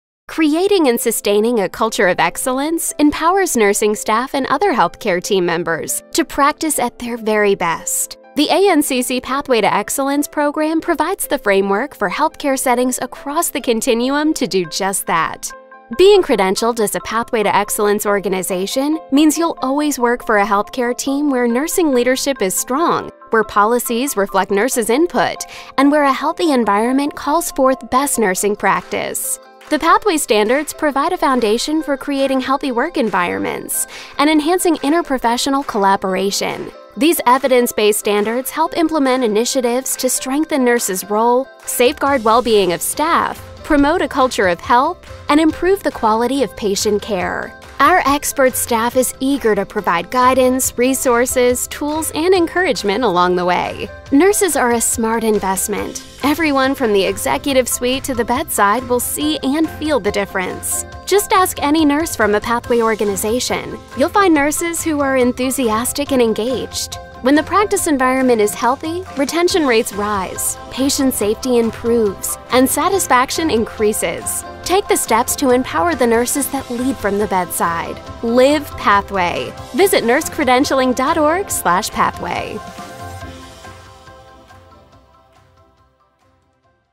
Introducing your Nursing Team to Pathway to Excellence
The Pathway to Excellence® Program recognizes health care and long term care organizations for positive practice environments where nurses excel. Learn more about the Pathway to Excellence program